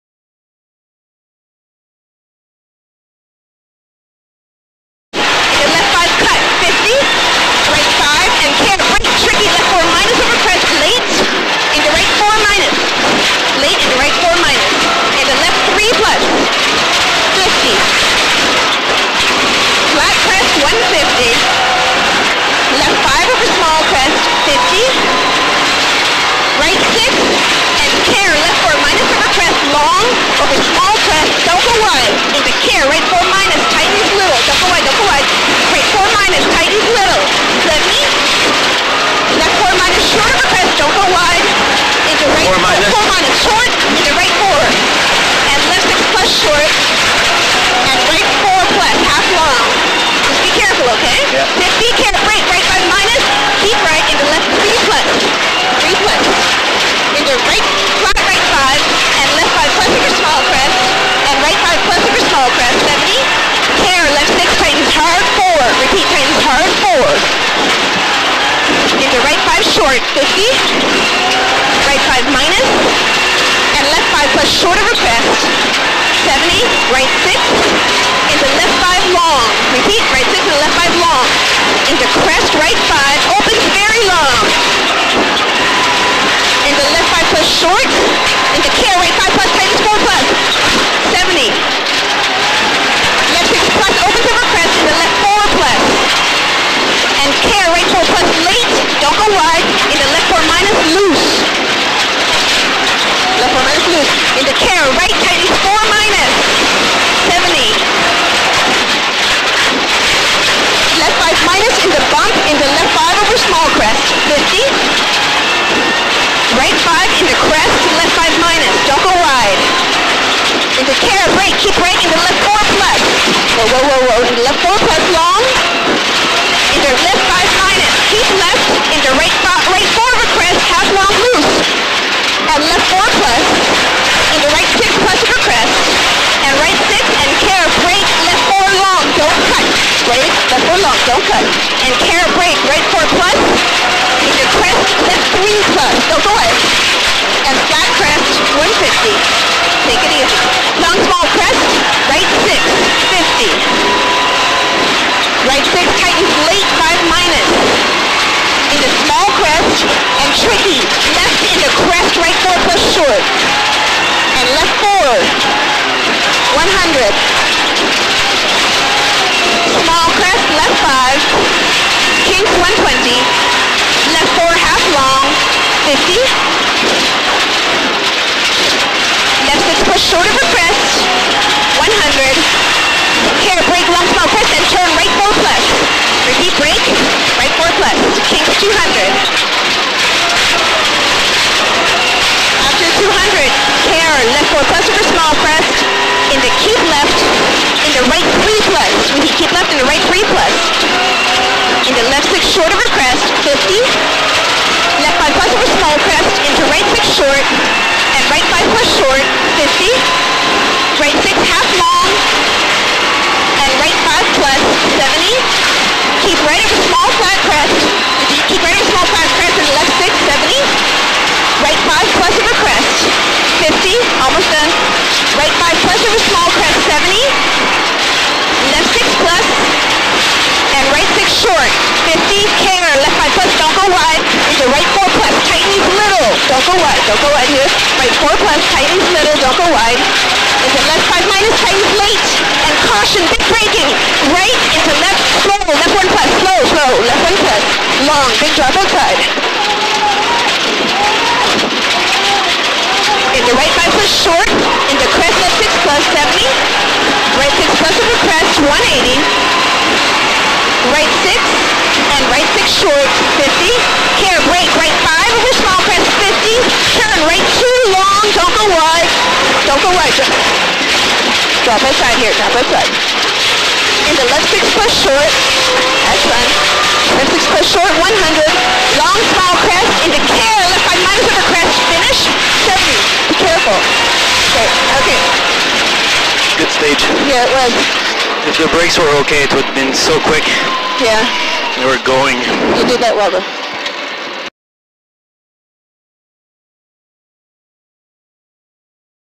Antoine L'Estage & Co-Driver Nathalie Richard at Speed
Rally American points leader Antoine L'Estage & co-driver Nathalie Richard at speed on special stage eight, Fir Mountain Long II, in the Oregon Trail Rally Saturday afternoon.